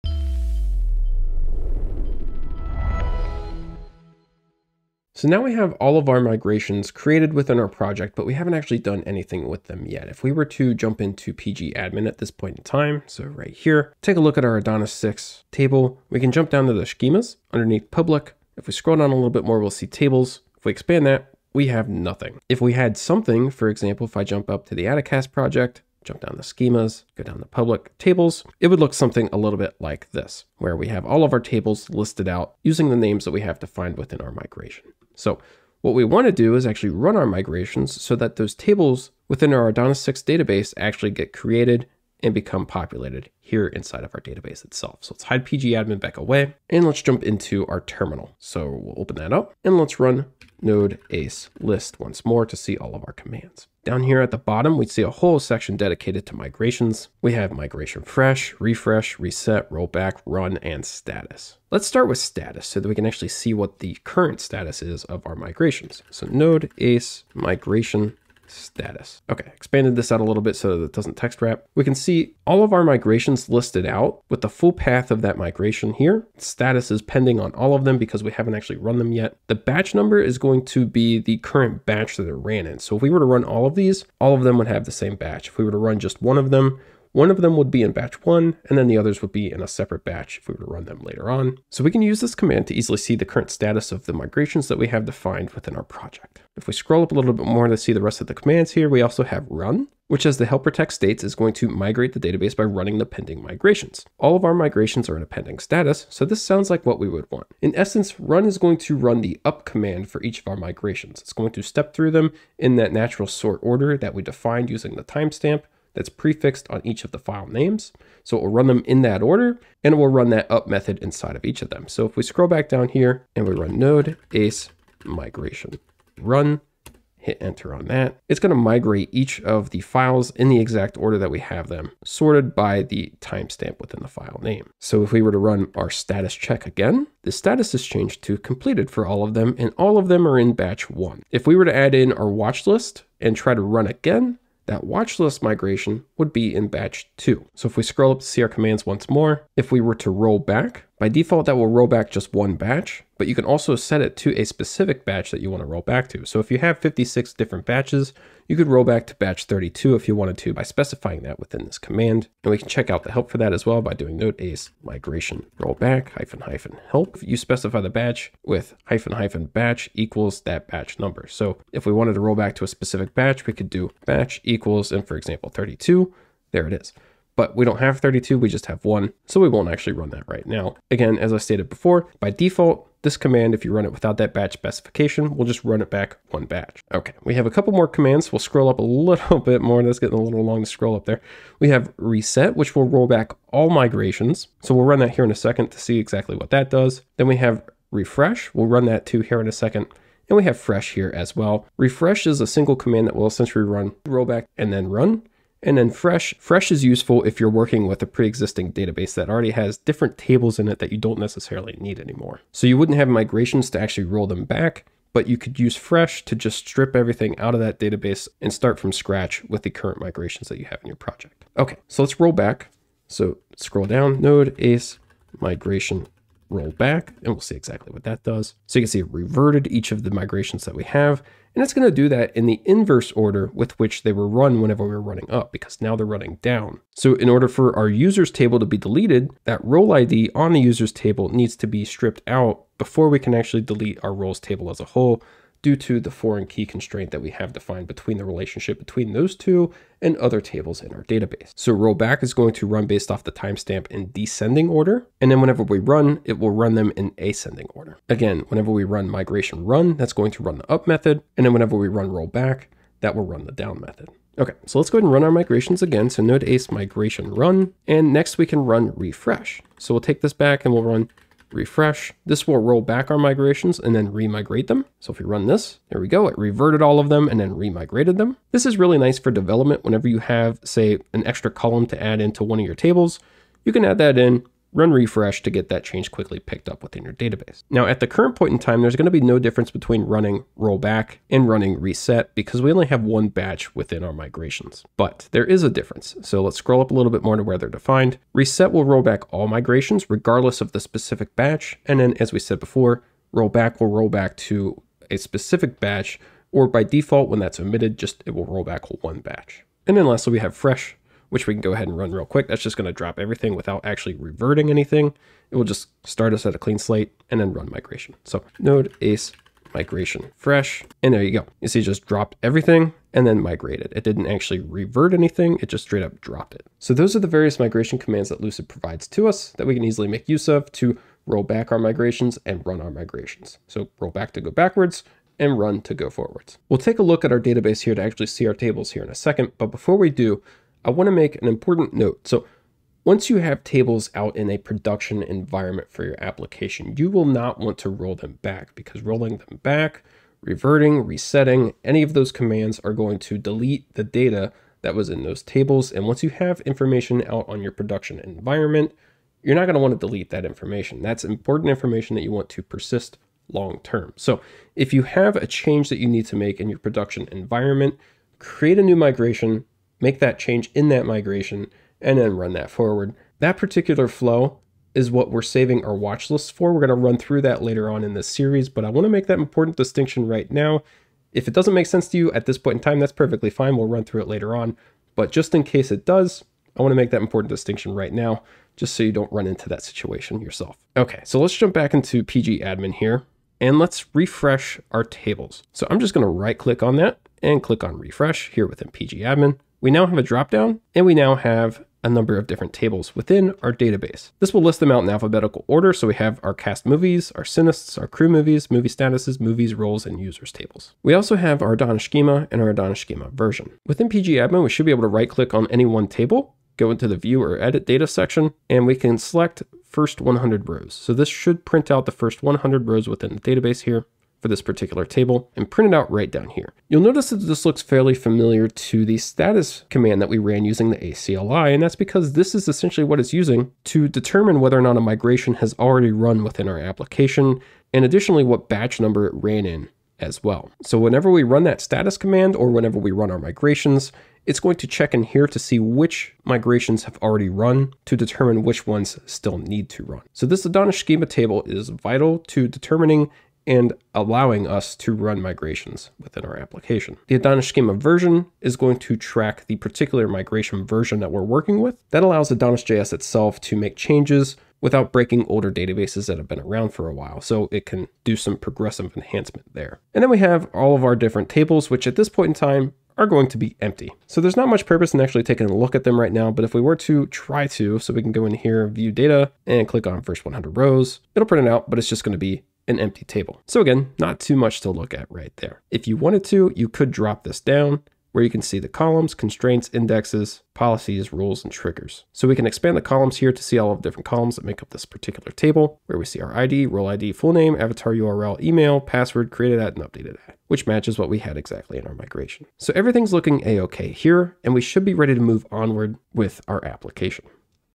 Let's Learn AdonisJS 6: Ep 4.3 - The Flow of Migrations
In this lesson, we'll learn about the various migration-based commands made available by the Ace CLI and how they interact with our migration files. We'll use these commands to run, rollback, and refresh our migrations against our database.

Socials